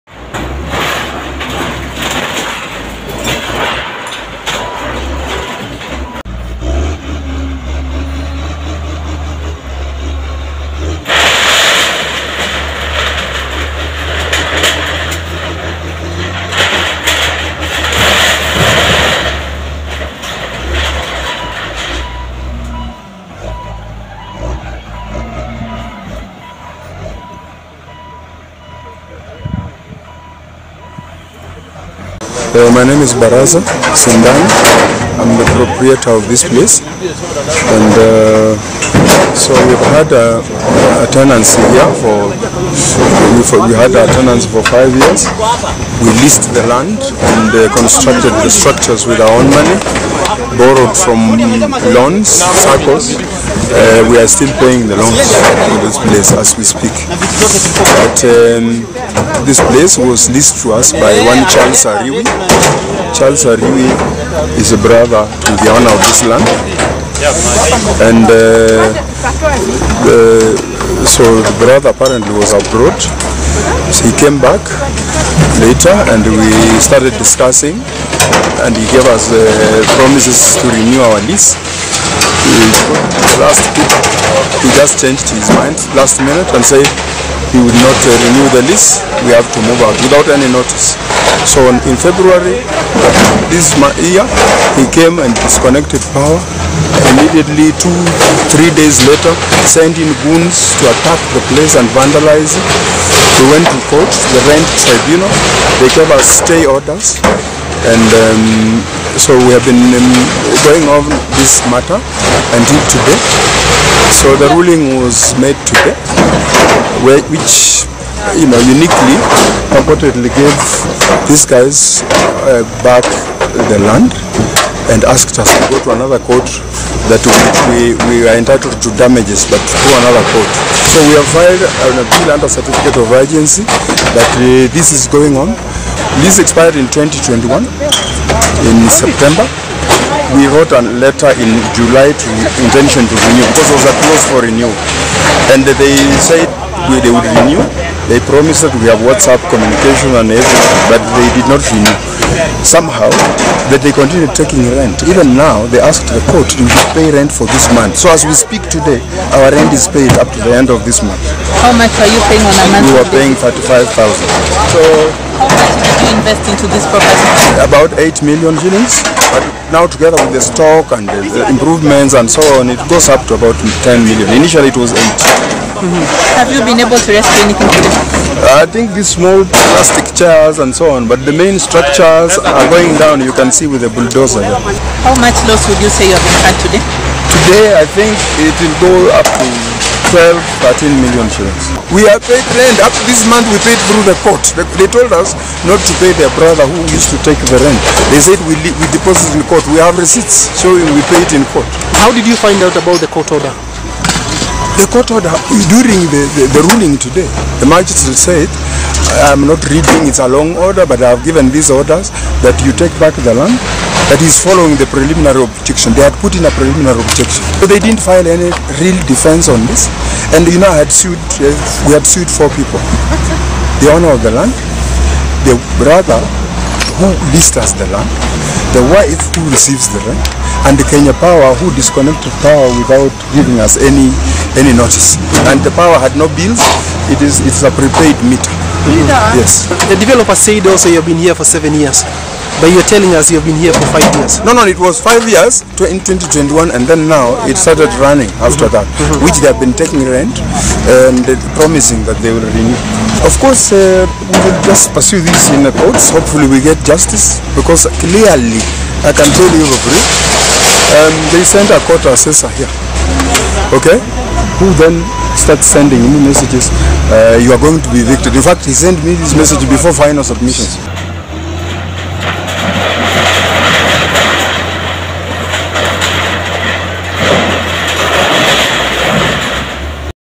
RONGAI RHUMBA HOUSE DEMOLISHED FOLLOWING LEASE DISPUTE
Rongai Rhumba House was demolished on Friday, 12 May 2023 following a court ruling that favoured the land owner.

Elveza Media spoke to a man who was supervising the demolition on behalf of the land owner.

He said  "The court strick out a case filed by the tenant.  The landowner was issued with a court order which is currently being executed"

The man further said  "the tenant has been very  rude to the landowner and was not remmiting rent on time"

"Who would allow anyone to occupy such a large portion of land without paying rent?  He asked

Watch the following video interview of the proprietor of Rongai Rhumba House giving his side of the story